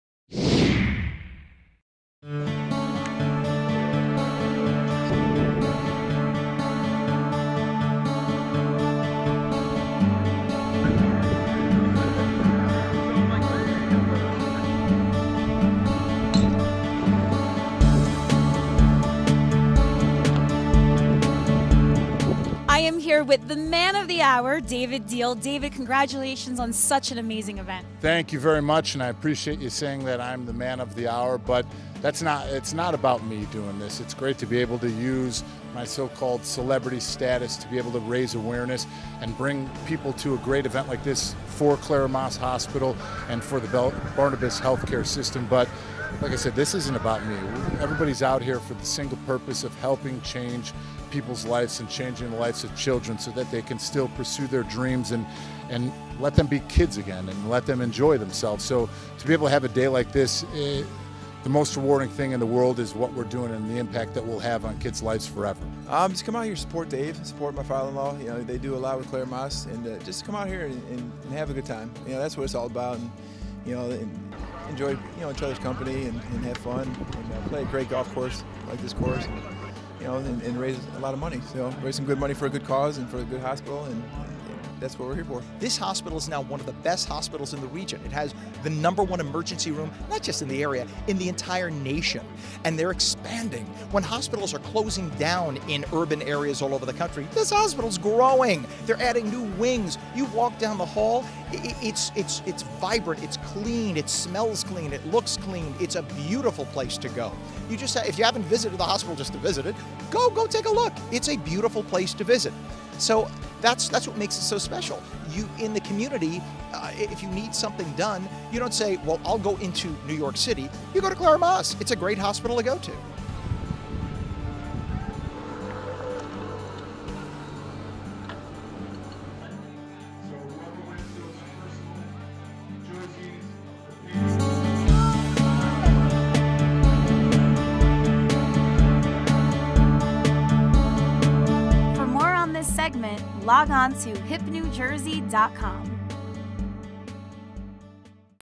Hip New Jersey - Clara Maass Medical Center Foundation Golf Invitational
Clara Maass Medical Center Foundation’s Golf Invitational took place at the Montclair Golf Club in West Orange on May 9. HipNewJersey was there to give you an inside look.

HipNJ’s Lisa Marie Latino had the privilege of speaking with the man of the hour, former New York Giant David Diehl, about the event. “This isn’t about me,” Diehl said. “Everyone is out here for the single purpose of helping change people’s lives and changing the lives of children so they could still pursue their dreams and let them be kids again.” Diehl went on to say that the most rewarding thing in the world is “the impact that we will have on kids' lives forever.”

Kevin Walter, former Houston Texan also came out to support. He expressed his excitement to “Have a good time, enjoy each other’s company, and raise money for a good cause and a good hospital.”

WABC anchor Ken Rosato expressed that Clara Maass Medical Center is “one of the best hospitals in the region.” It currently has the number one emergency room in the nation and is constantly expanding.

“When hospitals are closing down in urban areas all over the country, this hospital is growing,” Rosato explained.
He said that the hospital is a beautiful and vibrant place to go, encouraging those who have not yet seen it to visit.

“In the community, if you need something done, you go to Clara Maass,” Rosato stated. For more information on Clara Maass, head over to BarnabasHealth.org/ClaraMaass.